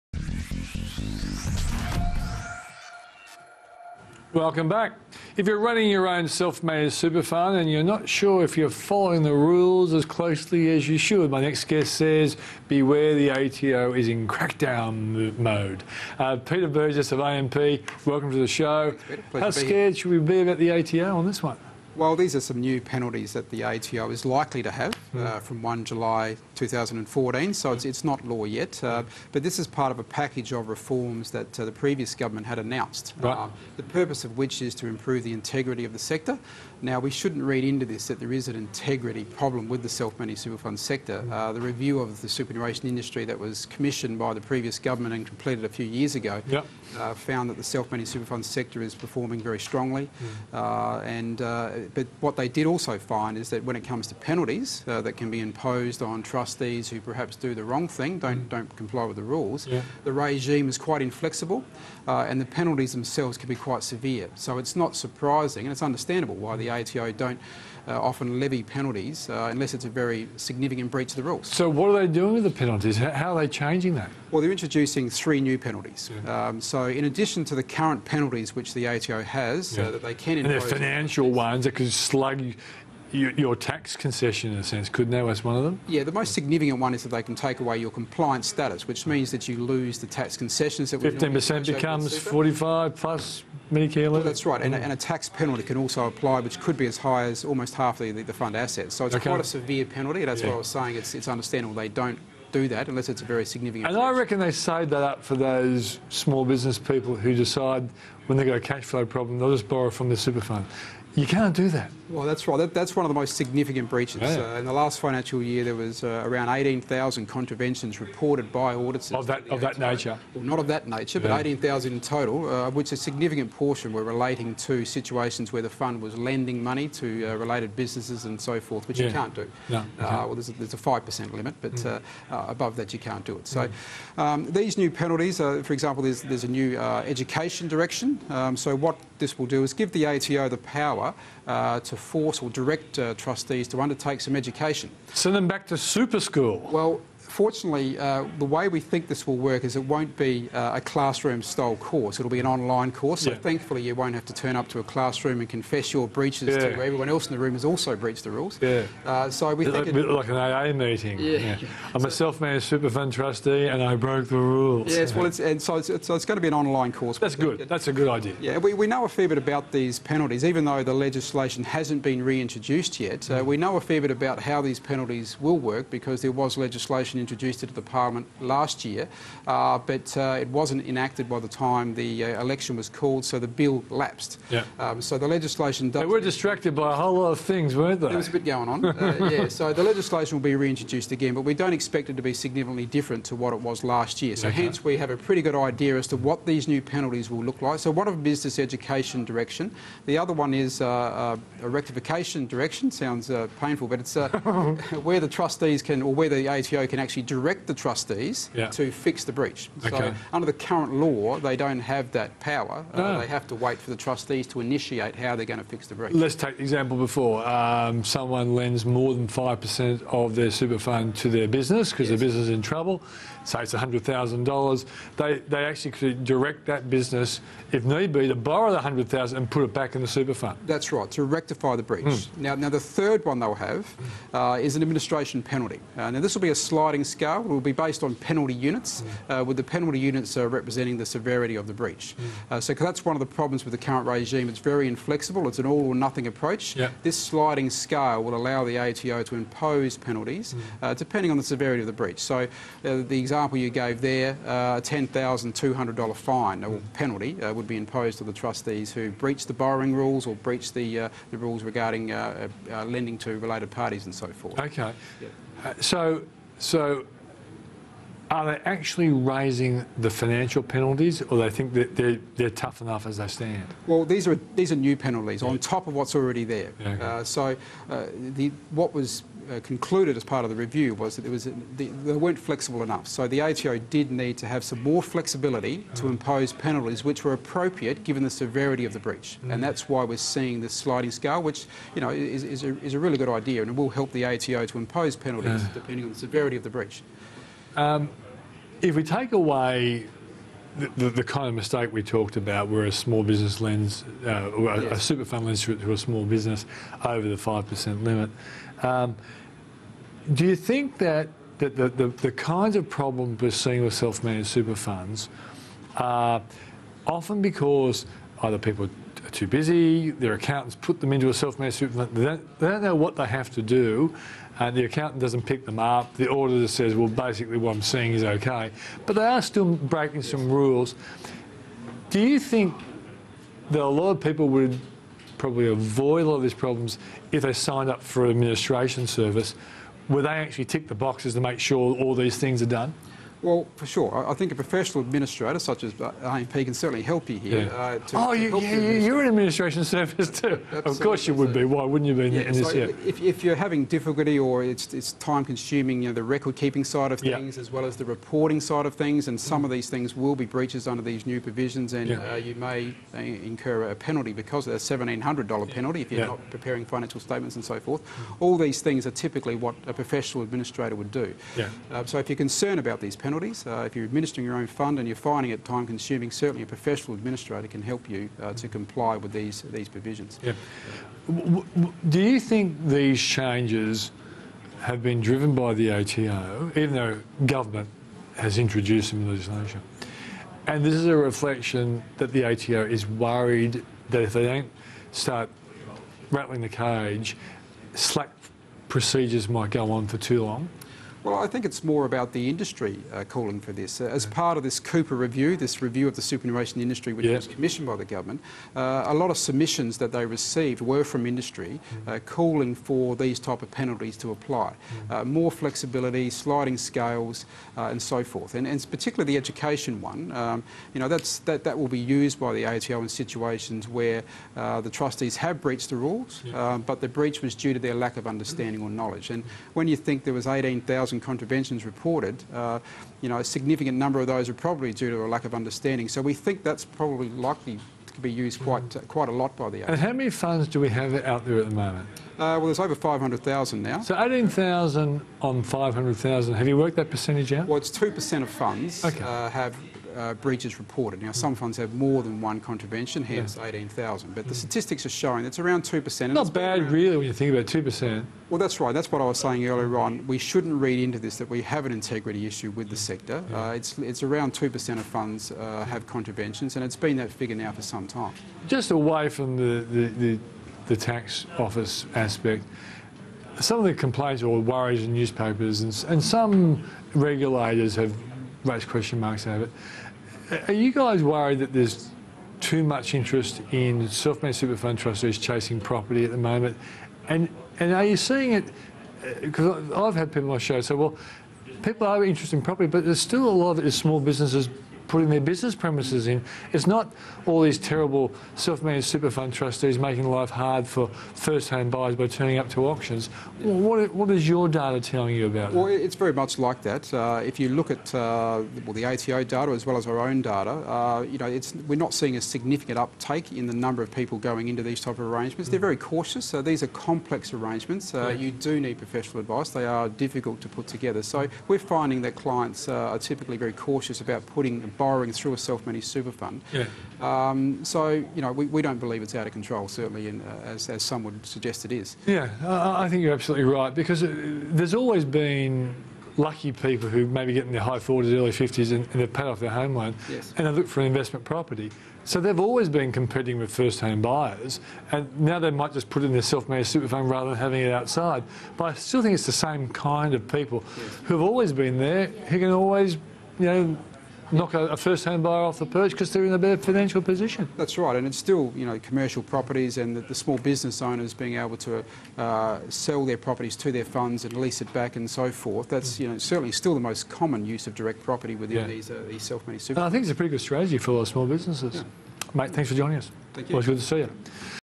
New ATO laws for SMSFs
With a string of new SMSF legislations and penalties introduced by the ATO recently, it's important trustees know where they stand - and what they can do to ensure they don't end up in hot water. To talk us through the ins and outs of the new rules, we speak with AMP SMSF's Peter Burgess.